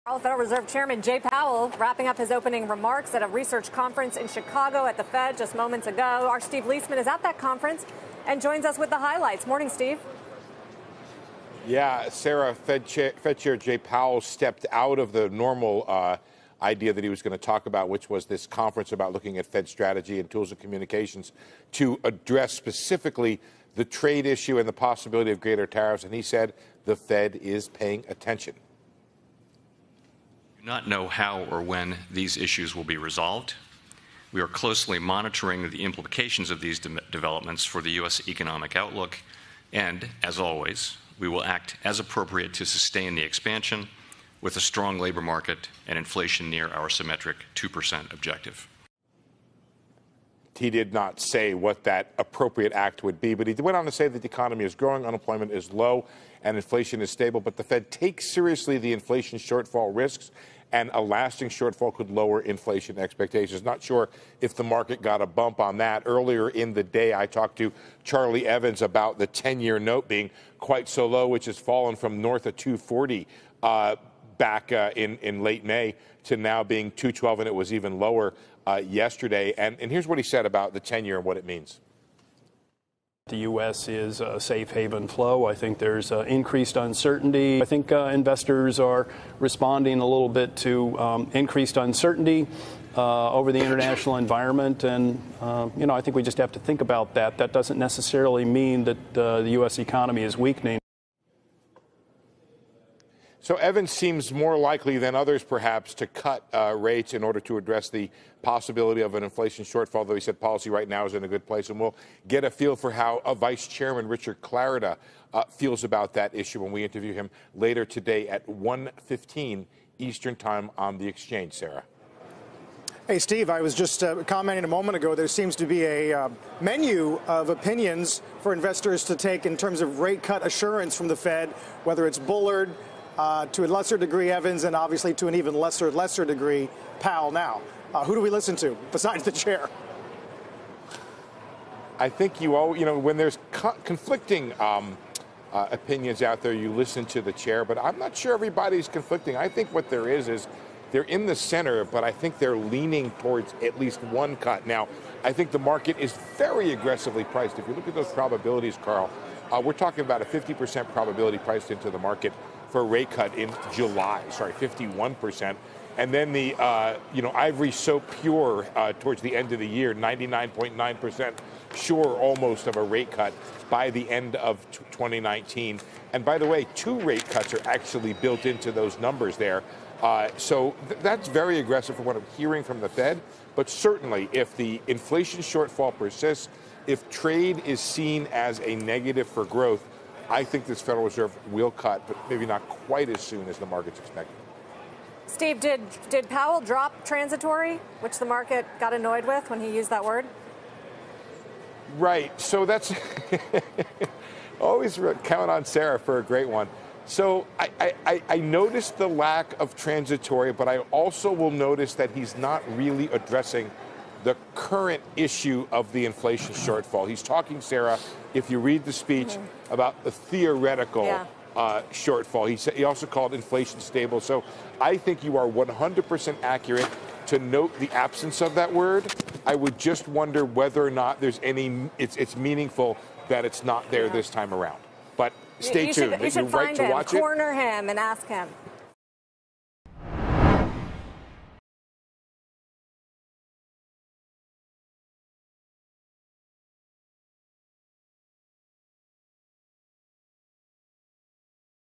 Fed Chair Powell: Economy growing, unemployment and inflation low
CNBC's Steve Liesman reports from Chicago, where Fed Chair Jerome Powell commented on the economy, trade and more.